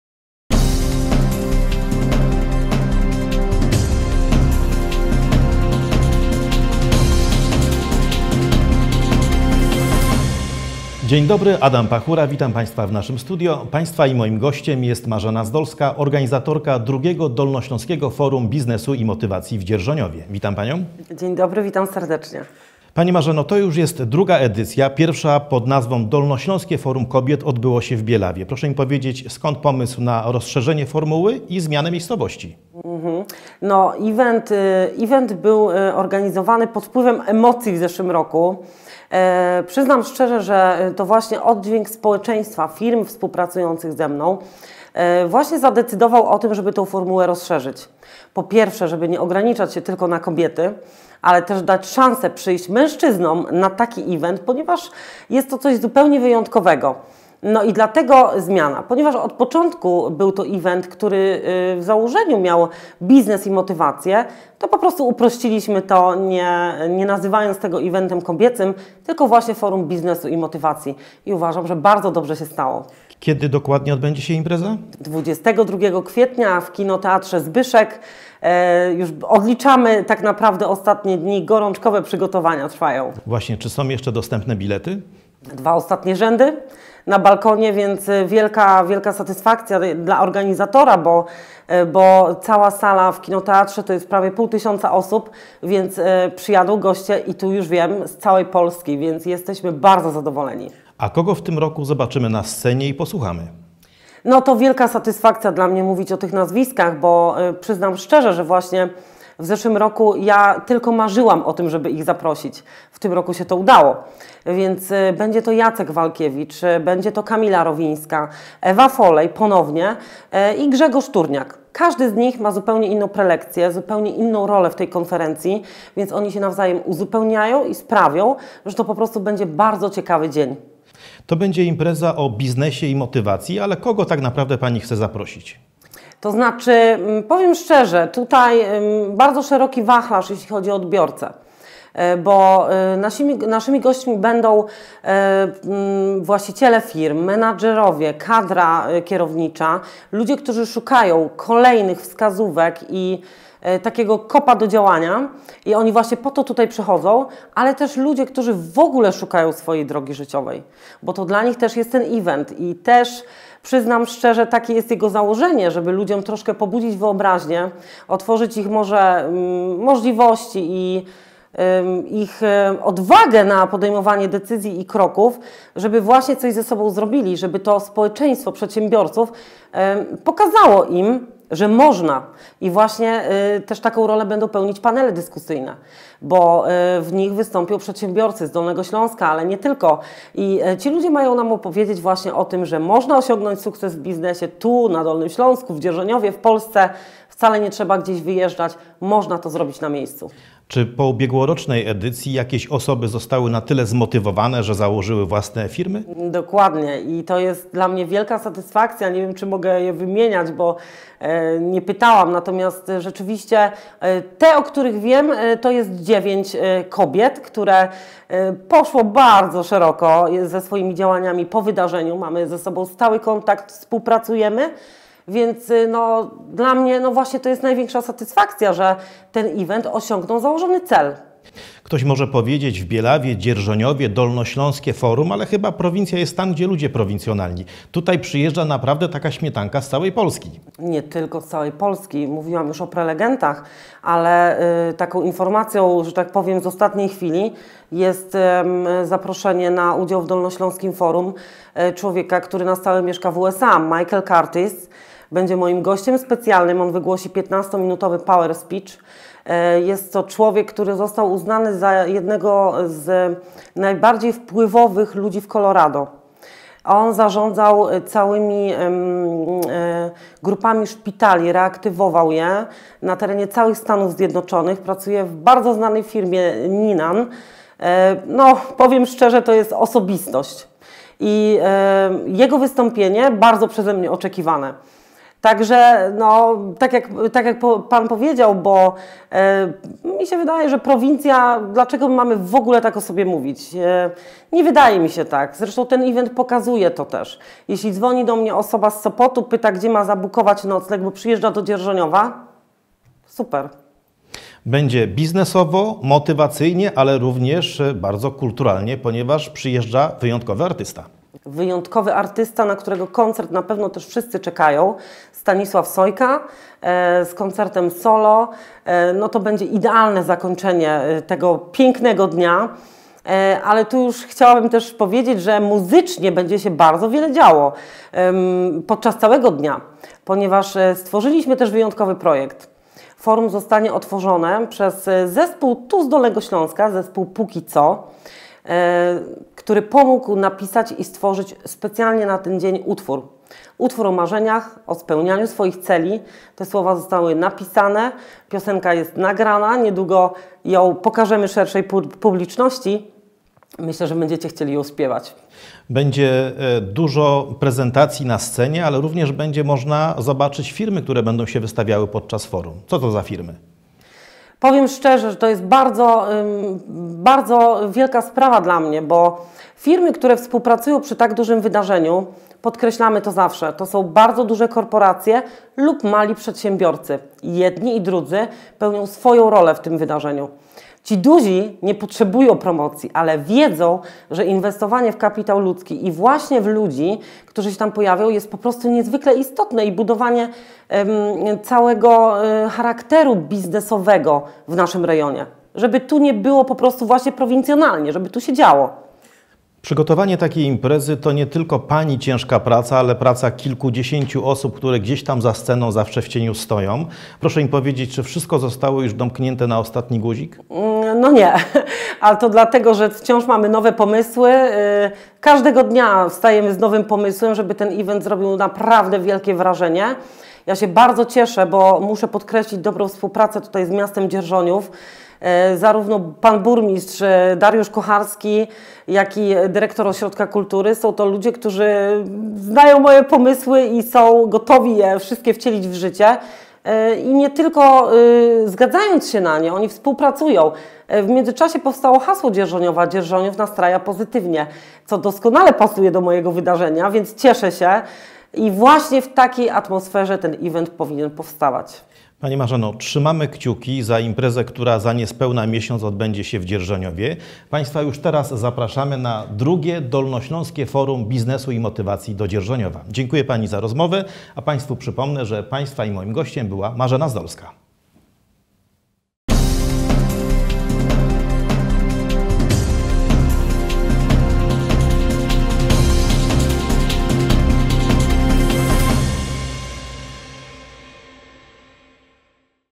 ROZMOWA Z MARZENĄ ZDOLSKĄ
Kolejnym gościem w naszym studio jest Marzena Zdolska, która opowiada o Dolnośląskim Forum Biznesu i Motywacji.